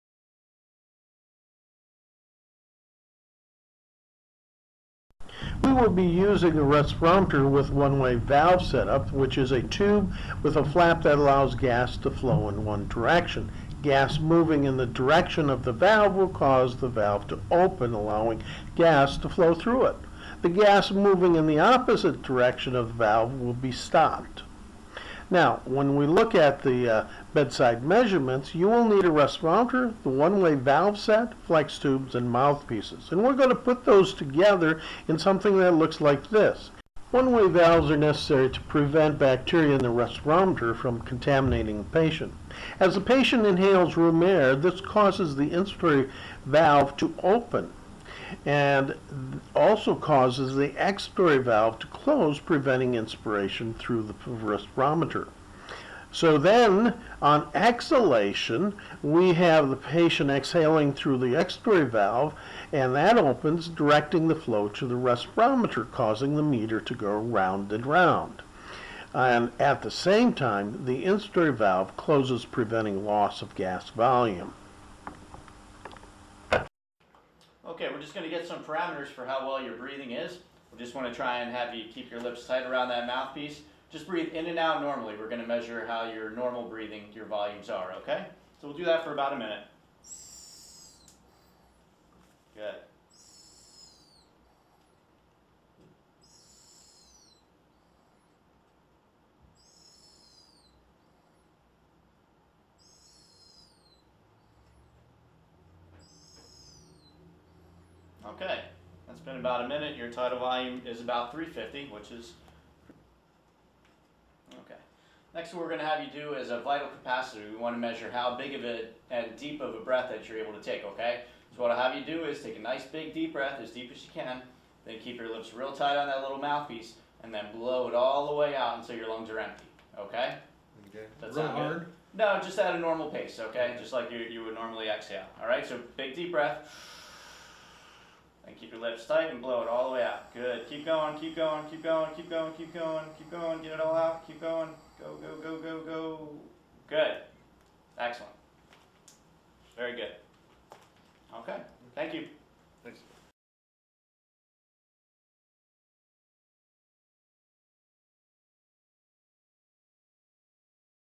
RESPIRATORY CARE: Bedside volume measurements
This video demonstrates the set up and procedures used with bedside measurements. This includes one way valve set up, minute volume, tidal volume and vital capacity.